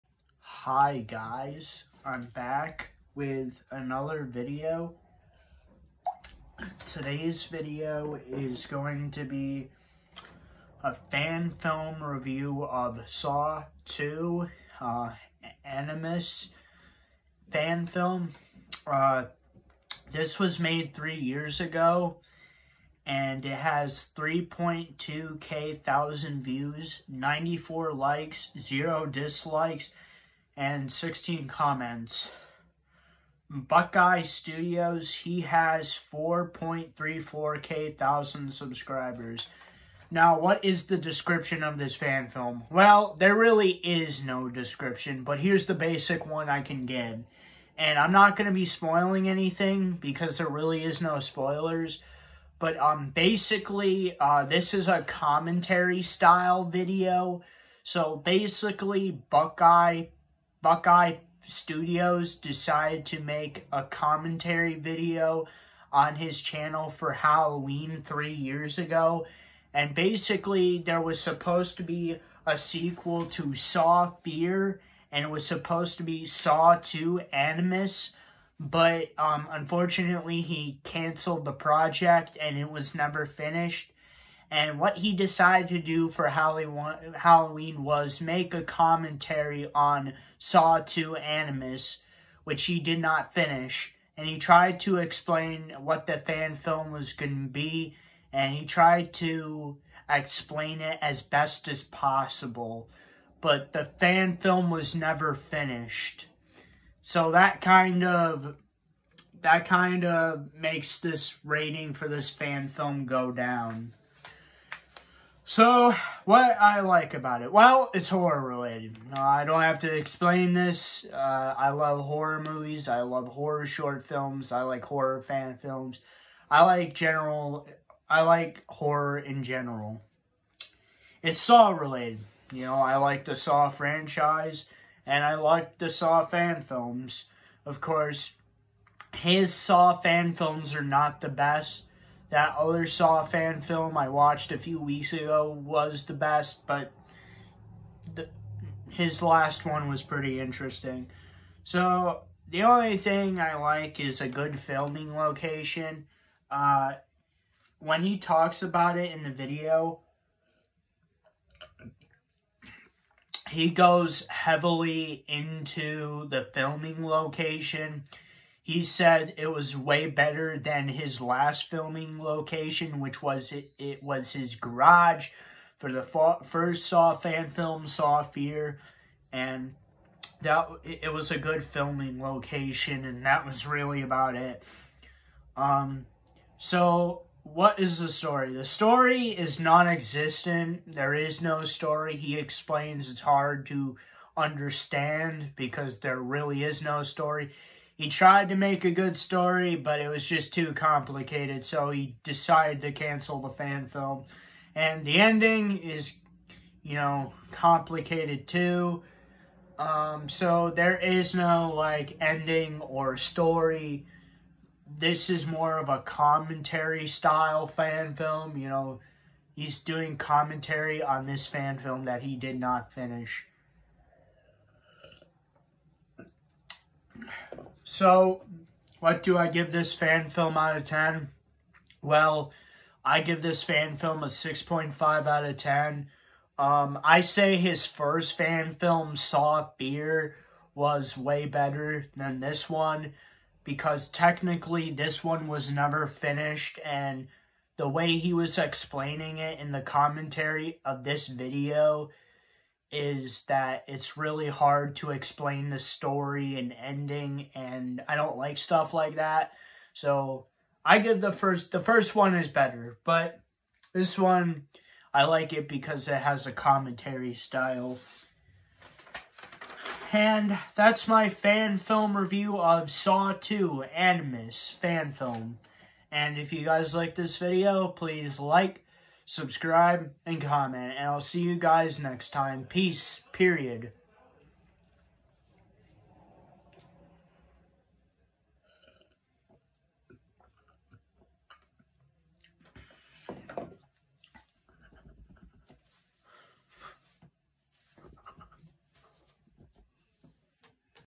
Fan Film Review Of Saw 2:Animus (Fan Film)
Hi guys, I’m back with another video. Today’s video is going to be a fan film review of saw 2:Animus (fan film) and this came out 3 years ago and it has 3.2 K thousand views 94 likes 0 16 comments buckeye studios he has 4.34K thousand subscribers there is no description and there really is no spoiler for this fan film because this fan film never really happened. I kinda explain it in the video but if you watch his video he explains it even more but this was originally that this was supposed to be a sequel to his original fan film saw:fear but it got canceled because of a over complicated story and he just decided to cancel this fan film. He goes more into the video he does about it. He actually decided to do it for Halloween because he wanted to make a Halloween video and this is what he could make and he made a commentary on his own fan film, watch his video for a better understanding. Other than that. What do I like about it while it’s horror related and it’s Saul related and it has a good filming location and that’s really about it. There really is no story. The story is nonexistent same thing with the ending. It’s nonexistent as well and if you watch buckeye studios his video, he explains it more than I do so go check out his video on this fan film and I will link in the description of this video for you guys to watch it so what do I get this fan so I’m out of 10 while I give it is 6.5 out of 10. I can tell you right now. His first fan film was way better. Of course it’s not the best. Saw fan film out there the saw sand film that I did a couple weeks ago was the best saw fan film on YouTube from what I’ve seen so go check that one out but that is my fan film review are the saw 2:Animus (fan film) and if you guys like this video, please like subscribe and comment and I’ll see you guys next time peace.